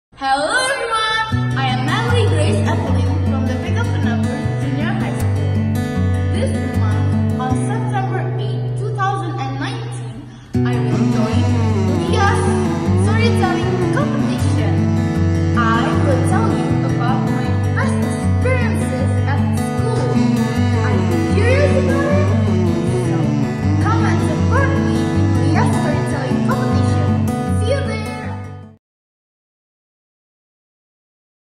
Story Telling - Natalie Grace.
LIA English Competition 2019.
No. Registrasi: 0010.
Nama peserta: Natalie Grace.
Sekolah: SMPK BPK Penabur.
Jenis Lomba: Story Telling
Dukung peserta dengan cara like video ini di akun instagram @lialampung 😊😊😊